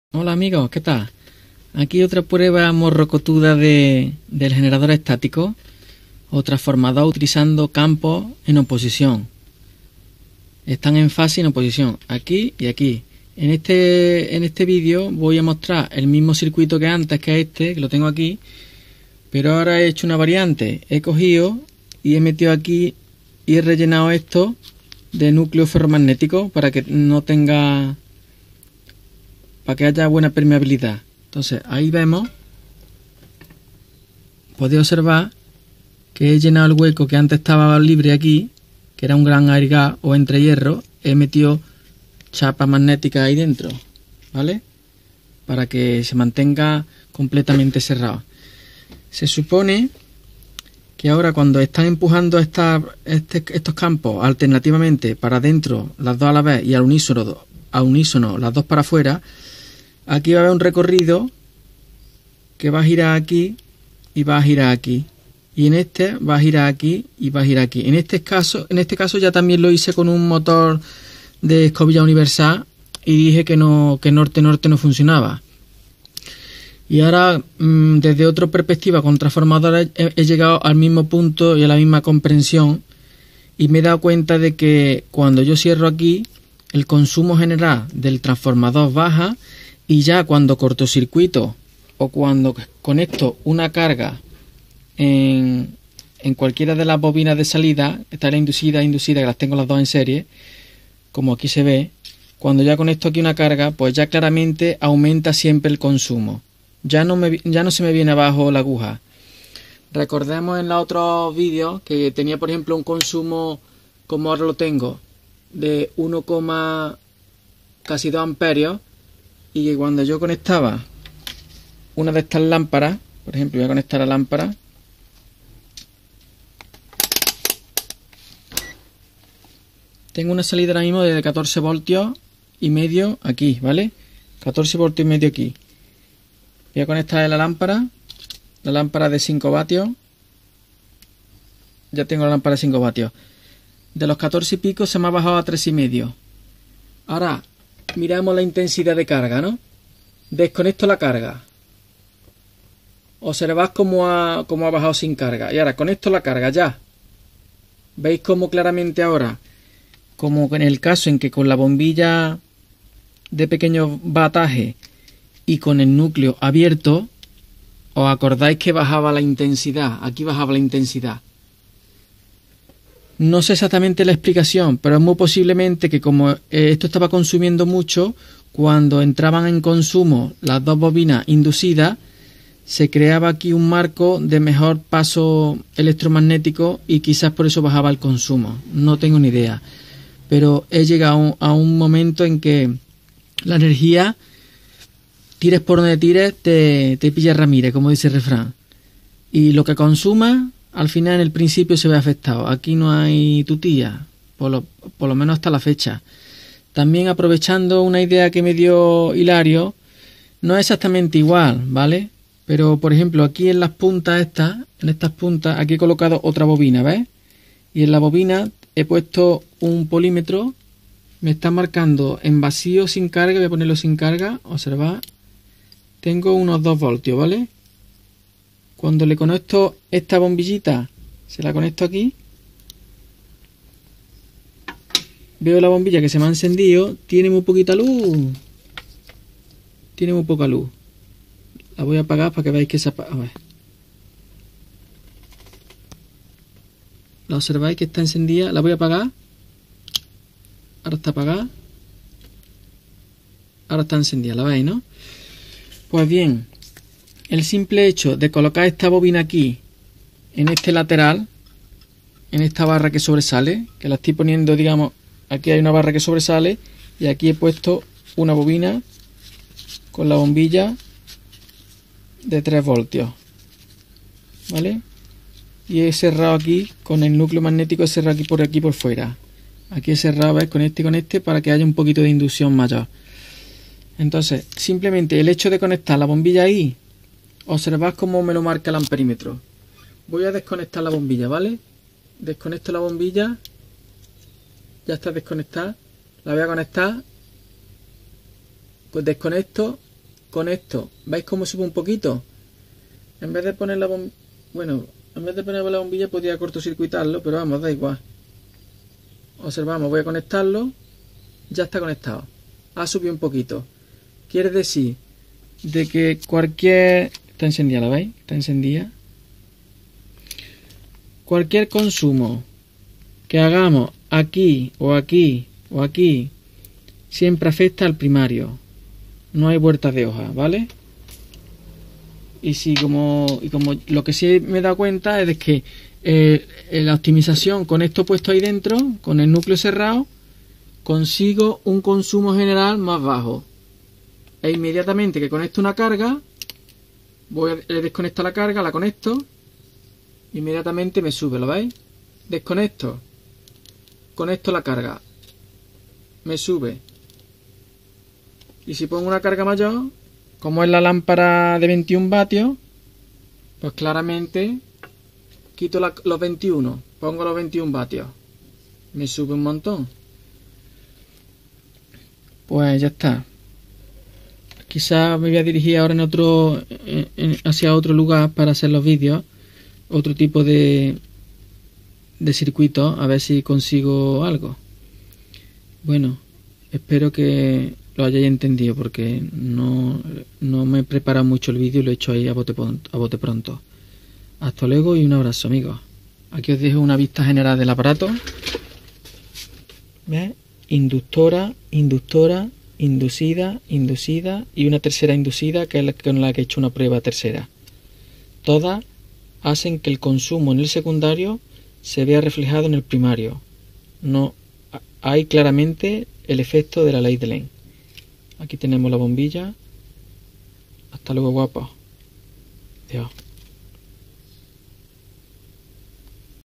Generador Estático C.A. 9°Parte.
Experimentos de electricidad y magnetismo.
Buscando un generador de electricidad que no sea transformadora de energía mecánica a eléctrica, sino una verdadera generatriz autoexcitada, sin gasto alguno y por tiempo ilimitado.,